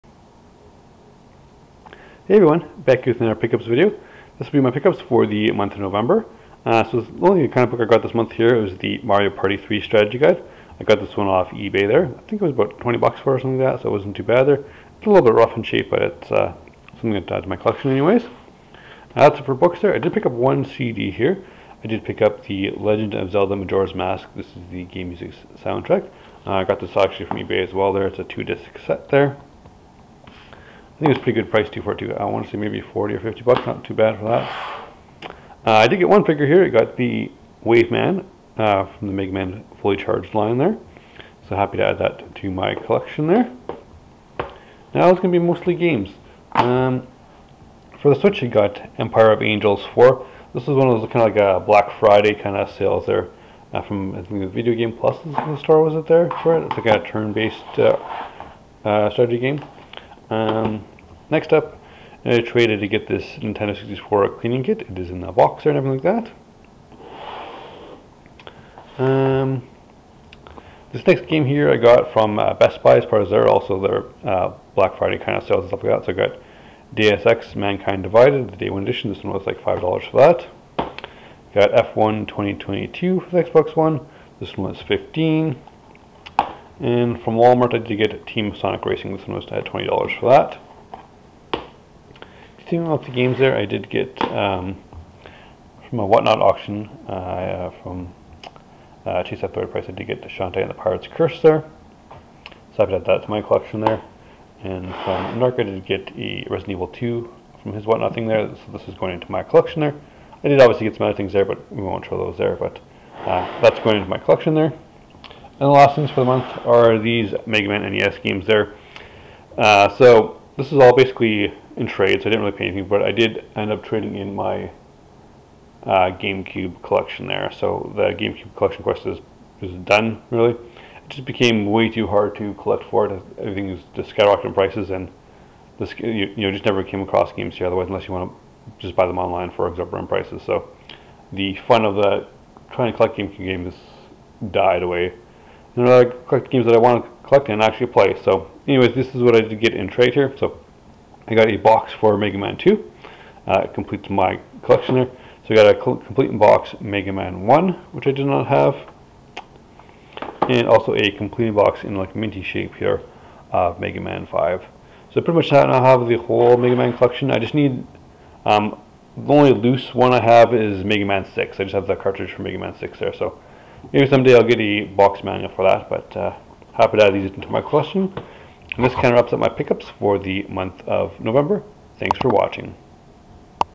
Pick Ups #167 - Mega Trade
Hey everyone, here are my pick ups from the month of November. Enjoy!